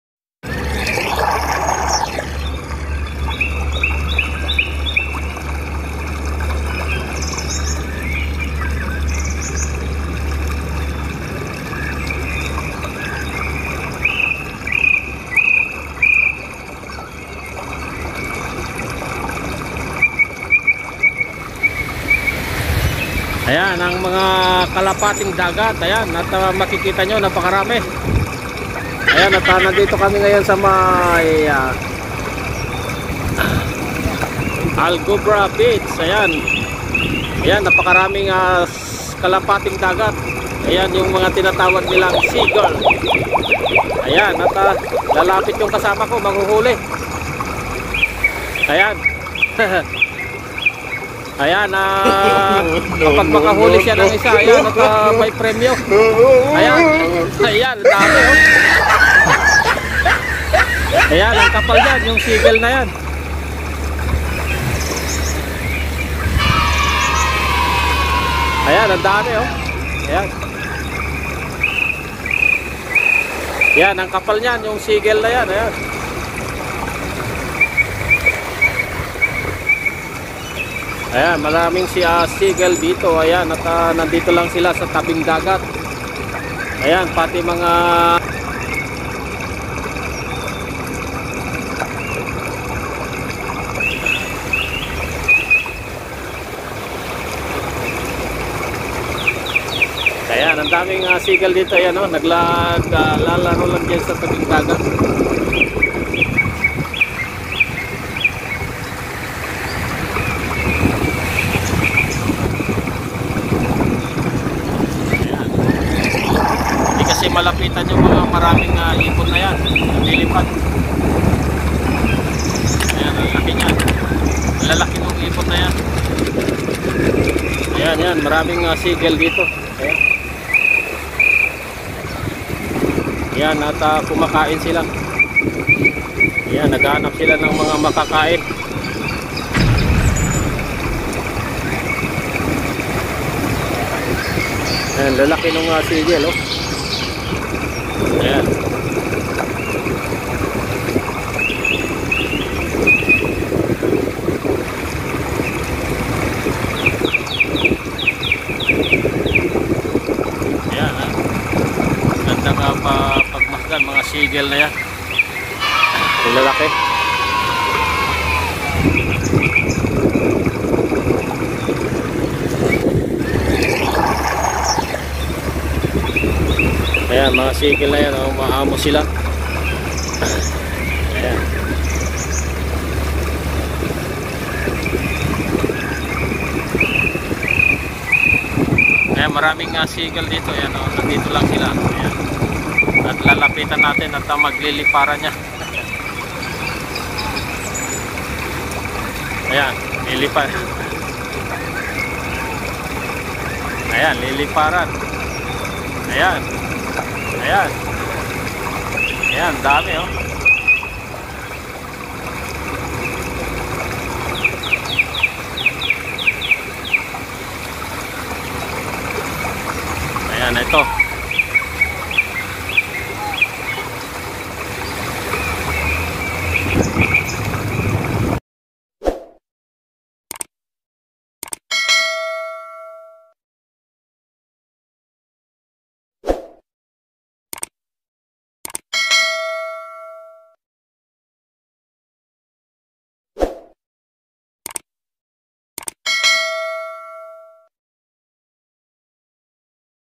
Sunset Gubrah Beach ll Walking and Watching Seagull Birds ll KUTING TV
Enjoy to watch my video.
Maganda maglakad lakad sa tabing dagat lalo na sa hapon at makita mo ang paglubog ng araw tapos nagliliparan na SEAGULL...